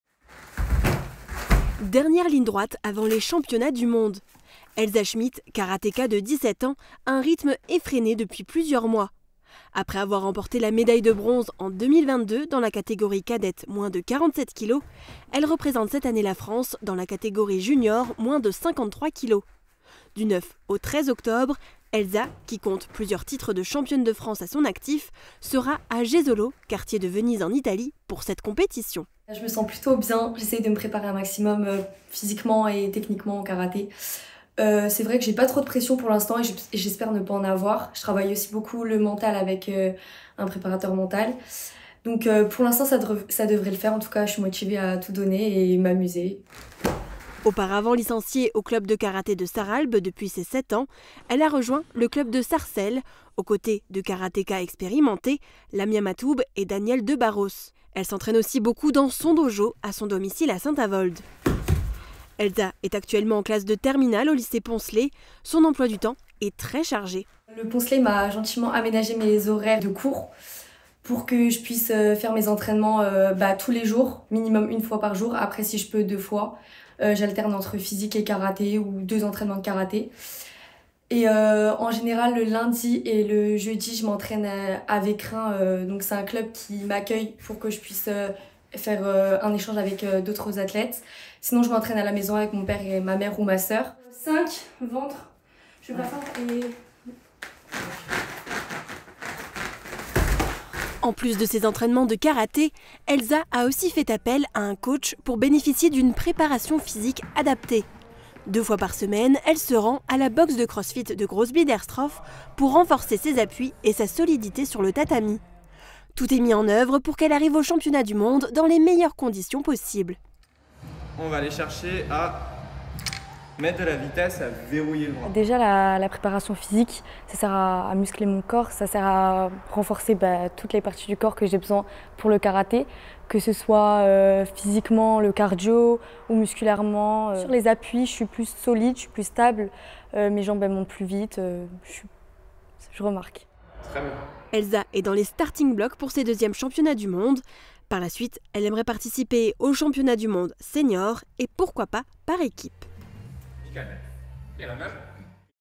Elsa Schmitt se prépare pour les championnats du monde de karaté !
Le quotidien de la lycéenne, habitante de Saint-Avold, s’articule entre ses cours et ses multiples entraînements, pour être prête physiquement et mentalement pour cette importante échéance.
Réalisation : Marie-Morgane PIRES